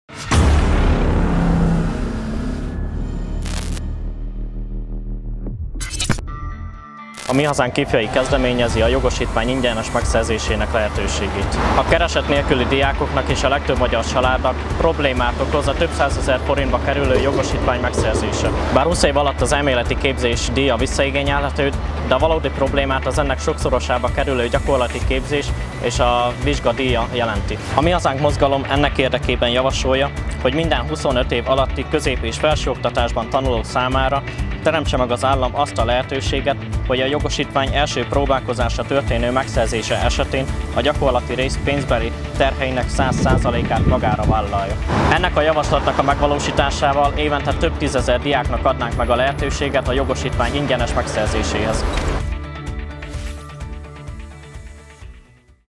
Ifjak Magyarországért: Ingyenes jogosítványt a diákoknak!
IFJAK MAGYARORSZÁGÉRT PROGRAM: Ingyenes jogosítványt a diákoknak!

A kereset nélküli diákoknak és a legtöbb magyar családnak problémát okoz a több százezer forintba kerülő jogosítvány megszerzése. Bár 20 év alatt az elméleti képzés (KRESZ) díja visszaigényelhető, de a valódi nehézséget az ennek sokszorosába kerülő gyakorlati képzés és vizsga megfizetése jelenti.

A Mi Hazánk Mozgalom ennek érdekében javasolja, hogy minden 25 év alatti közép- és felsőoktatásban tanuló számára teremtse meg az állam azt a lehetőséget, hogy a jogosítvány első próbálkozásra történő megszerzése esetén a gyakorlati rész pénzbeli terheinek 100%-át magára vállalja. Ennek a javaslatnak a megvalósításával évente több tízezer diáknak adnánk meg a lehetőséget a jogosítvány ingyenes megszerzéséhez.